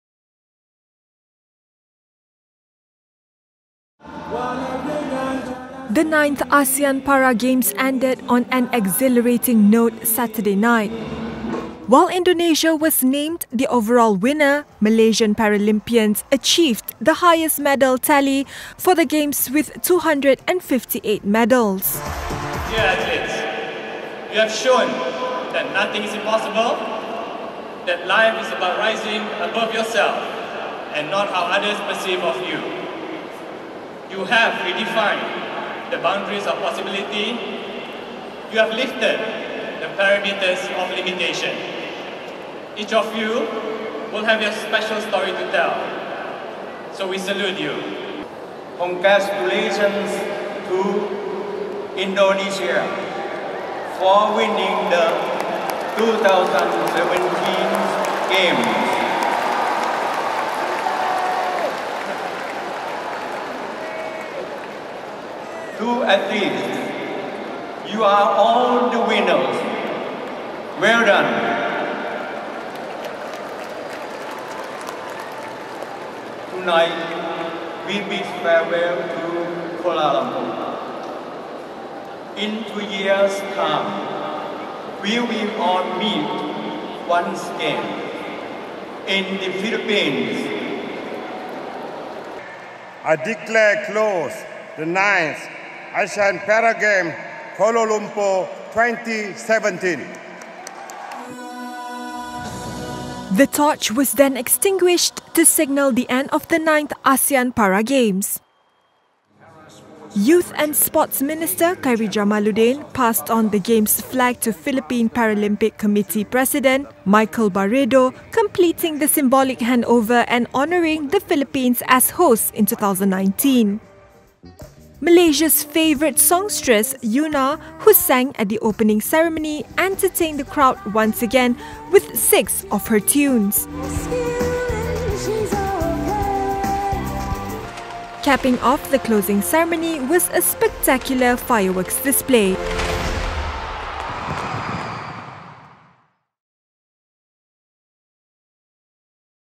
9th Asean Para Games ends with spectacular closing ceremony
The 9th Asean Para Games ended at the National Stadium in Bukit Jalil on Saturday with a spectacular closing ceremony and the official handover to 2019 hosts, the Philippines.